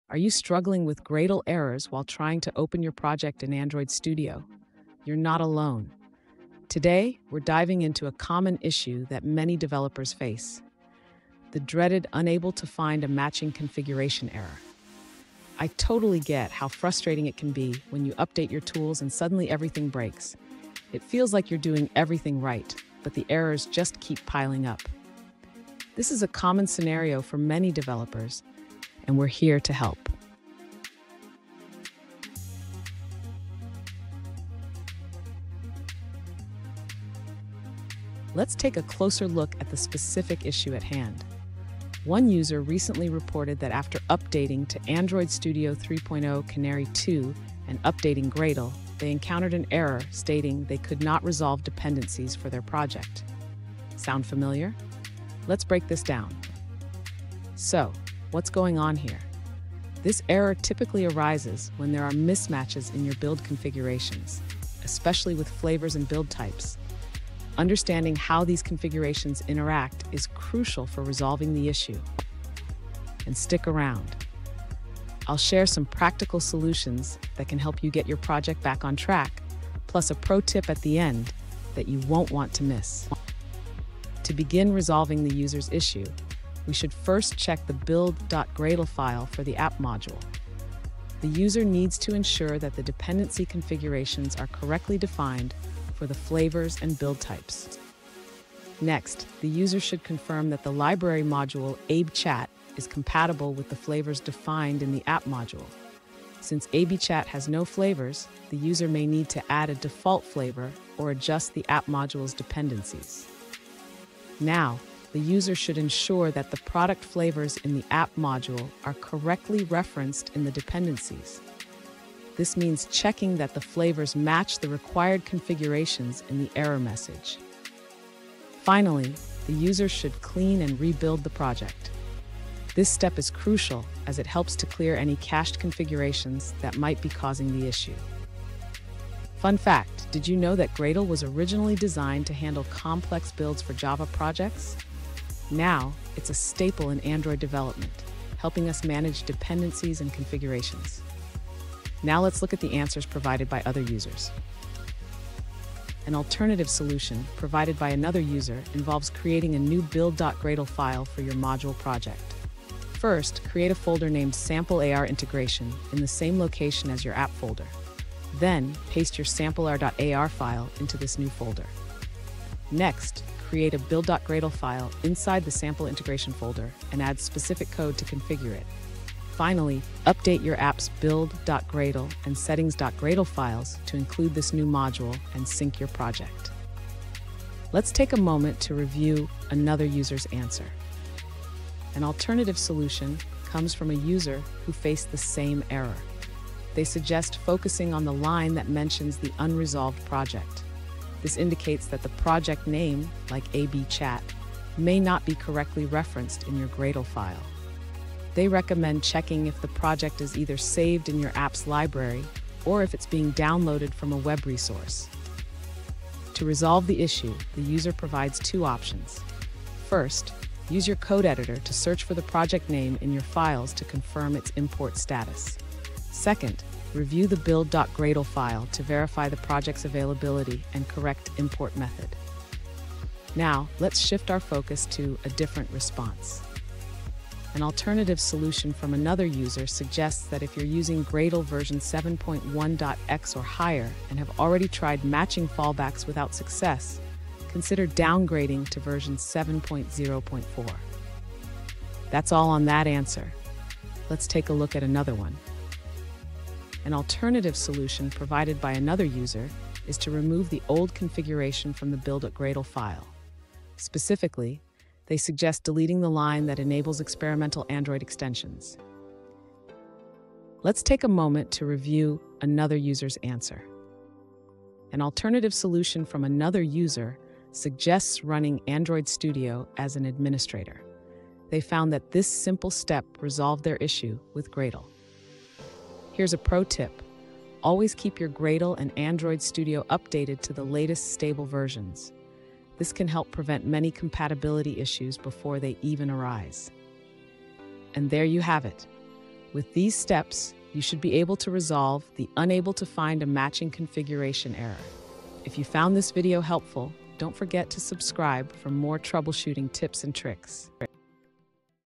Gradle 4.0 Unable to Find Matching Configuration: Troubleshooting Guide
In this video, we dive into a common issue faced by developers using Gradle 4.0: the "Unable to Find Matching Configuration" error. Whether you're working on a new project or maintaining an existing one, this troubleshooting guide will help you understand the root causes of this error and provide practical solutions to resolve it. Join us as we explore step-by-step methods to get your Gradle builds back on track!

Today's Topic: Gradle 4.0 Unable to Find Matching Configuration: Troubleshooting Guide

Related to